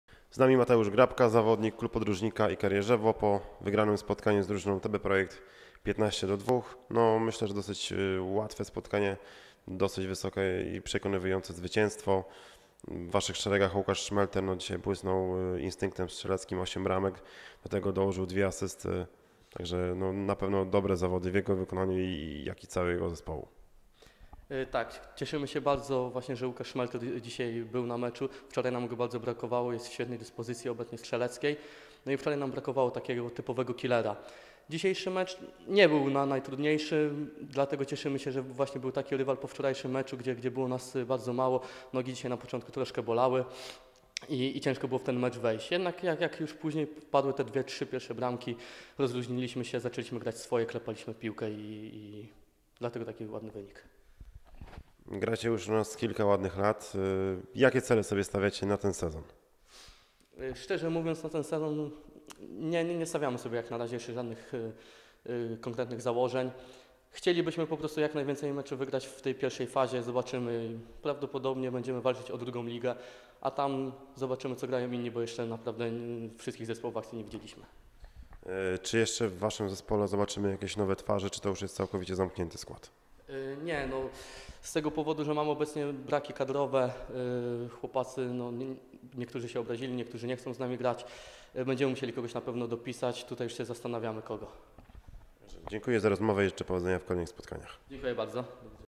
Wywiad z Mateuszem Grabką KP Ikar Jeżewo po wygranym meczu z TB Projekt 15-2
VI edycja HLPN "Stalex Liga" w Świeciu. Wywiad z Mateuszem Grabką KP Ikar Jeżewo po wygranym meczu z TB Projekt 15-2